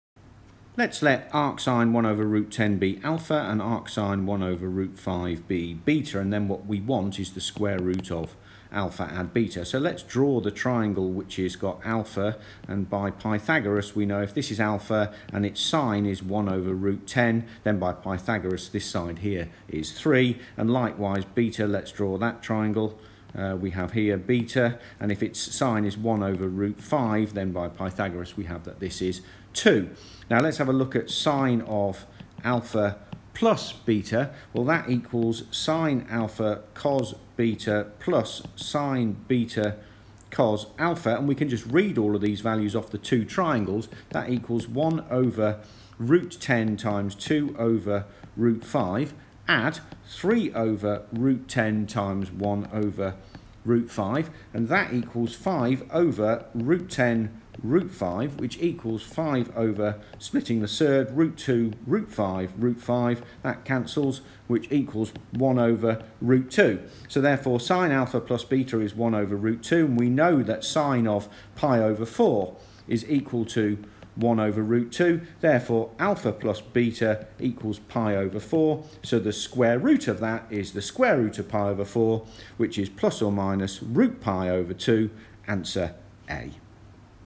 Two Arcsin Under a Square Root - Double Ouch!
Gresty Academy One Minute Teaser #131: It is not at all clear how to solve this lovely trig/square root teaser, but once the solution is revealed it is really very cool - did you get it correct?